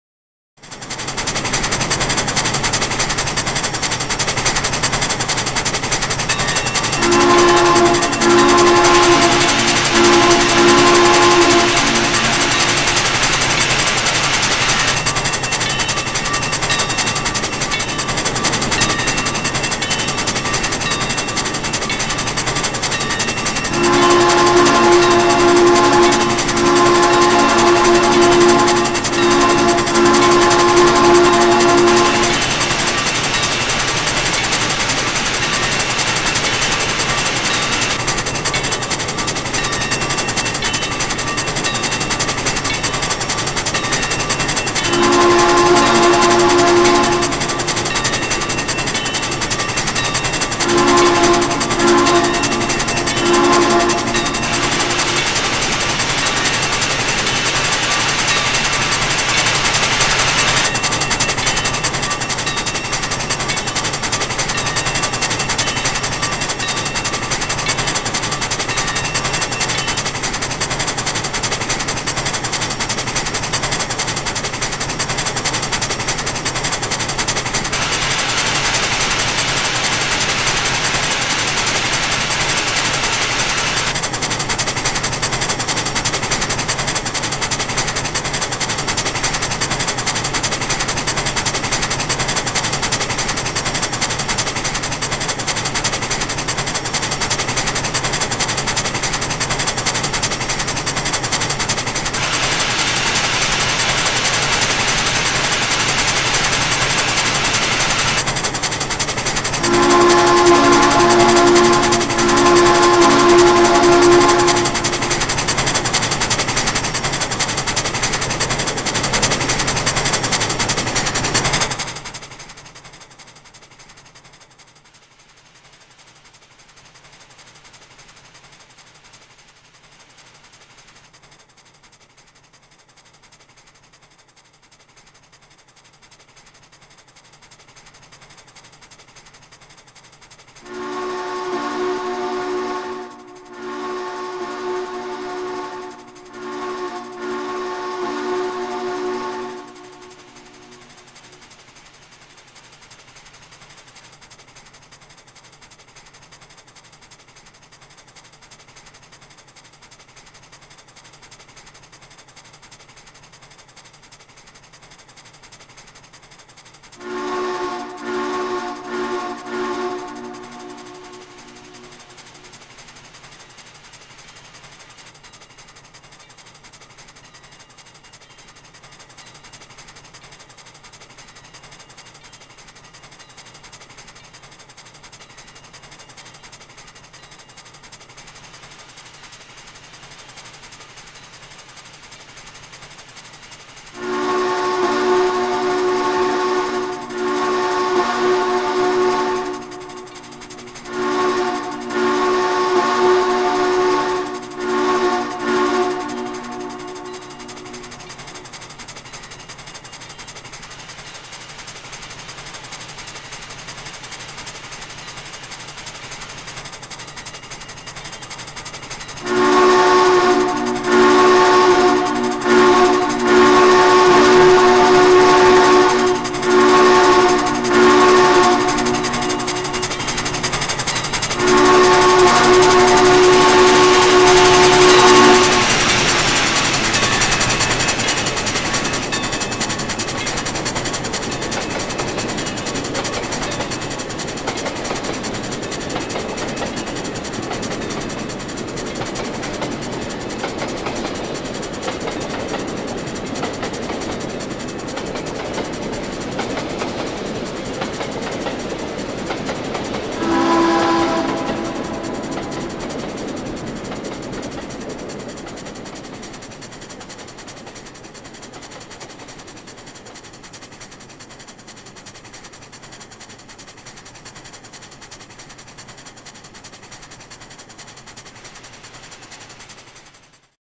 Frisco 1522's Epic Adventure Day 65 Part 3
After passing under the road bridge... Frisco 1522, her Auxiliary Water Tender, and her 18 Car Consist continue onward on the rollercoaster profile as she rounds more curves before heading onto a straight stretch. :)